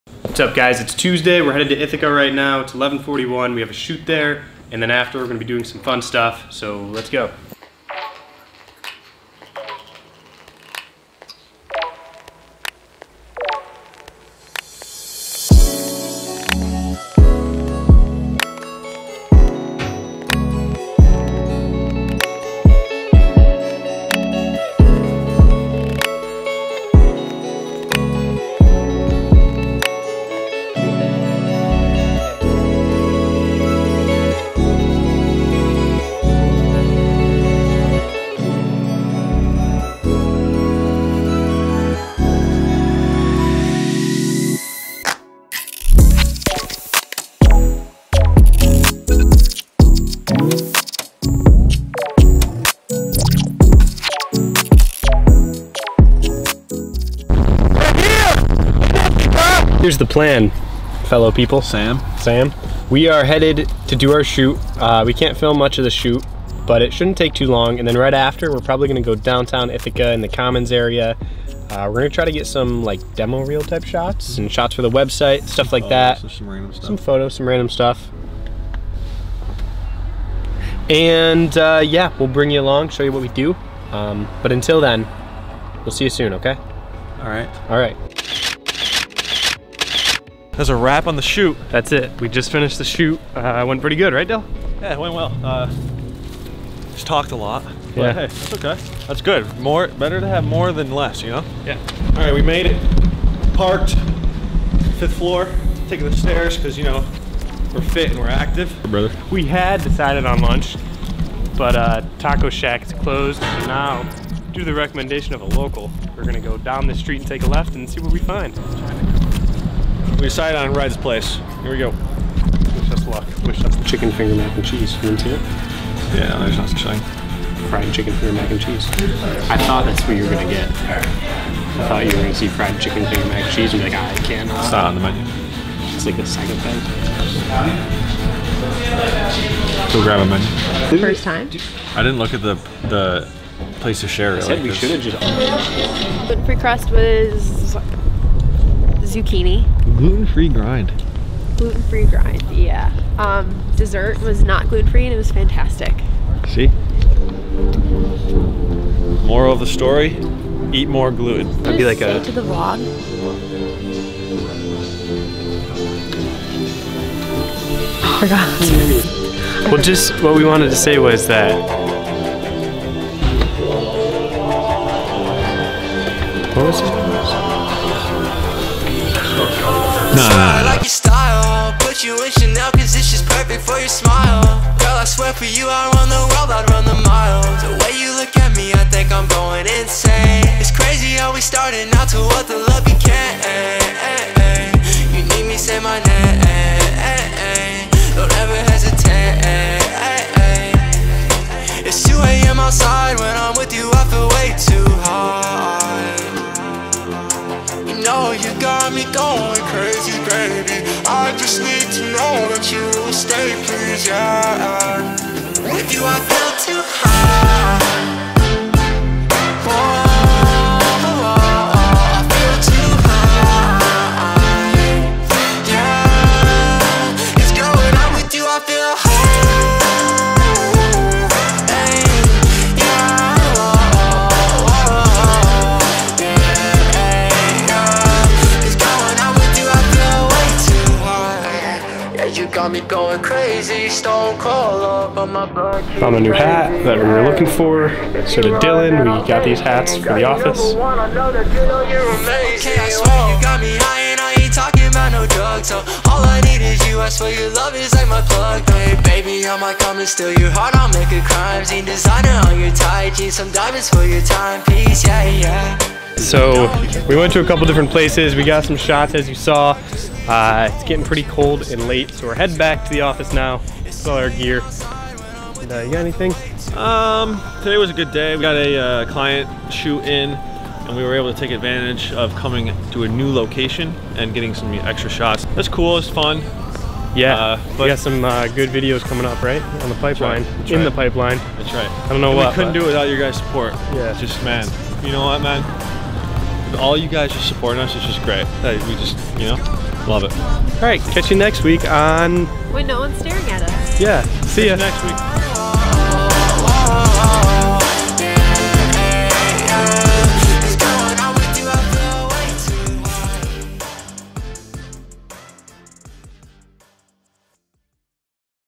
Vlogging Ithaca
We had a commercial shoot in Ithaca, NY so we decided to spend the afternoon exploring and checking out some local spots.